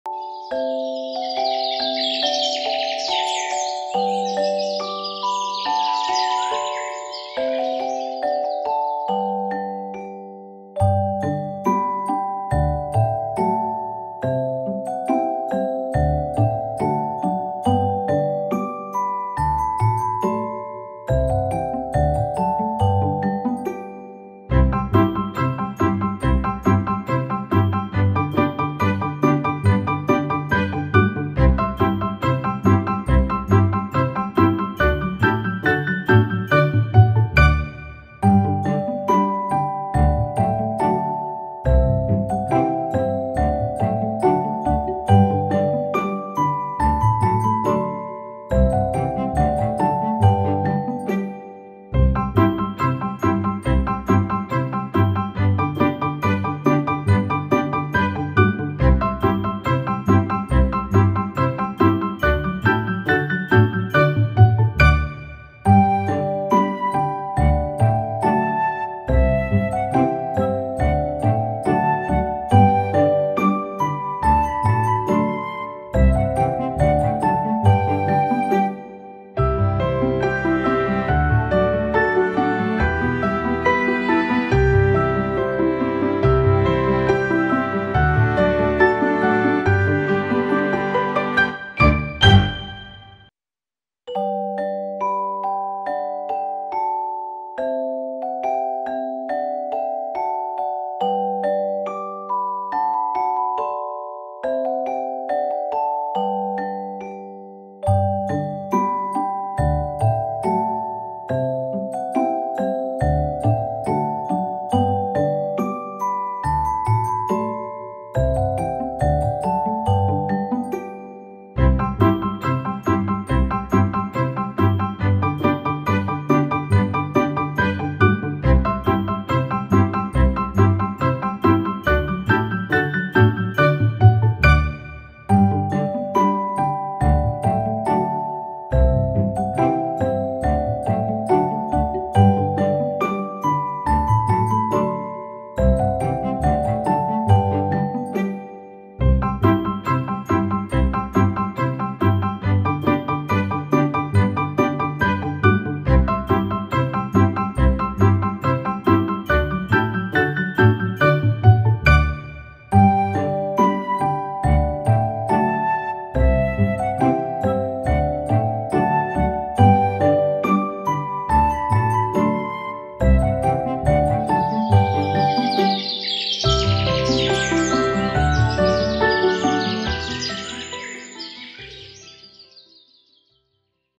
How to Drawing and Coloring Peppa Pig with cute Bird | Drawings For Kids
In this video, we will see how to colour a Peppa Pig 🐷 using colour Sketches. I hope you like this video❤️

Other impressive videos from our collections

THANKS ❤️
Sounds, Stock Video, Image & Vector Credits :
Pixabay.com | Vecteezy.com | Freepik.com | Pinterest.com